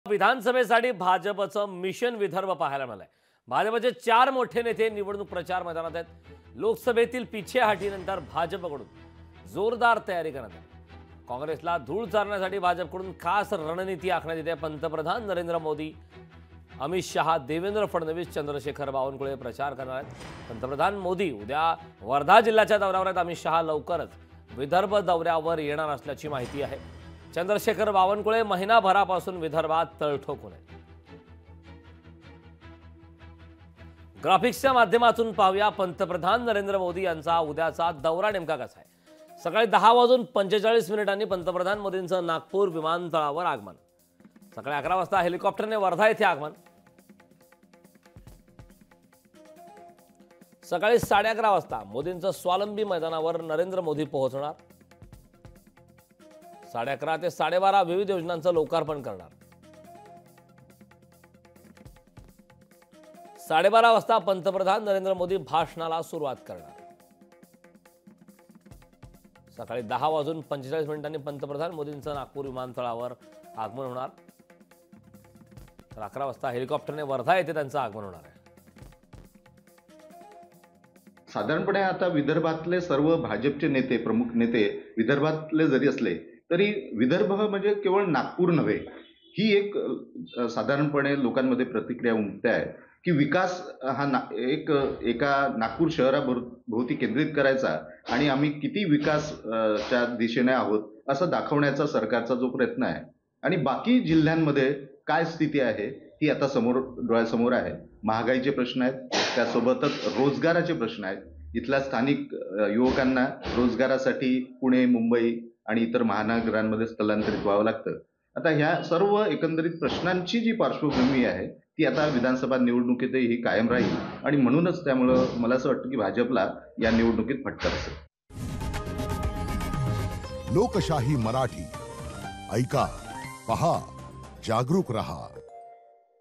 भाजपचे 4 मोठे नेते निवडणूक प्रचार मैदानात? | Marathi News
विधानसभेसाठी भाजपचे 4 मोठे नेते निवडणूक प्रचारासाठी मैदानात उतरणार आहेत.. लोकसभेतील पिछेहाटीनंतर भाजपकडून जोरदार तयारी करण्यात आलीय.. पंतप्रधान मोदी, शाह, फडणवीस, बावनकुळे प्रचार करणार आहेत..

4 big leaders of BJP are going to enter the field for the election campaign for the Legislative Assembly.. After the setback in the Lok Sabha, BJP has made strong preparations.. Prime Minister Modi, Shah, Fadnavis, Bawankule are going to campaign..